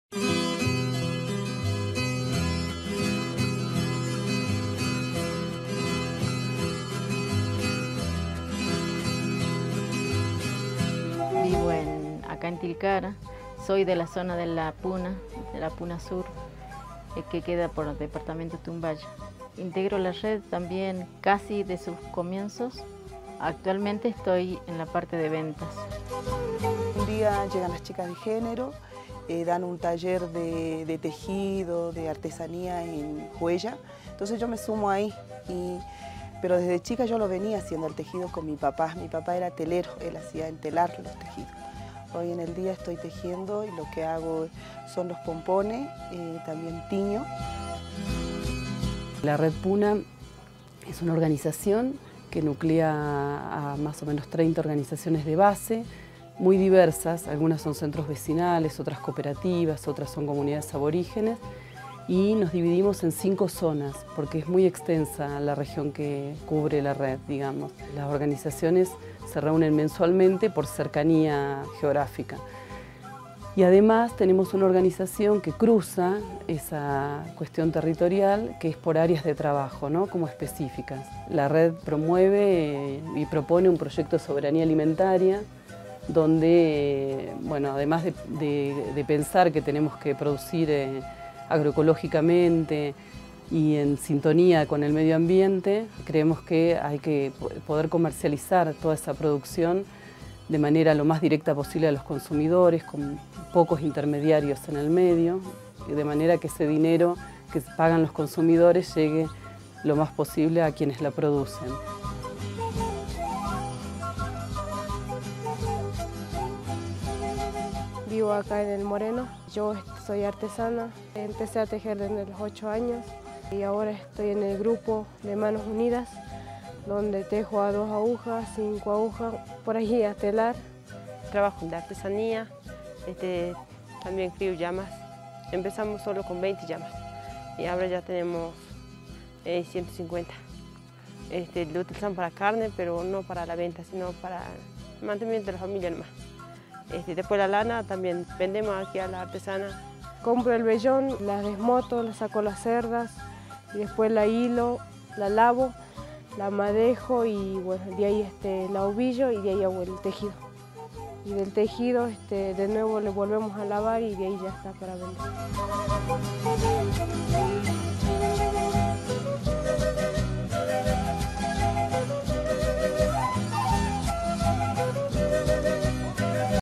RED PUNA parte 1
Llegamos hasta la provincia de Jujuy para conocer a una red de organizaciones que busca cambiar la transformar la sociedad. Se llama Red Puna y agrupa a comunidades aborígenes, campesinos, artesanas, mujeres y jóvenes.
La Red Puna fue fundada en 1995.está integrada por más de 30 organizaciones de comunidades aborígenes, campesinos, artesanas, mujeres y jóvenes. Se realizan artesanías con materiales originarios y técnicas ancestrales. El proceso consta de la crianza y esquila de animales, la fabricación del hilo y el tejido para lograr los productos.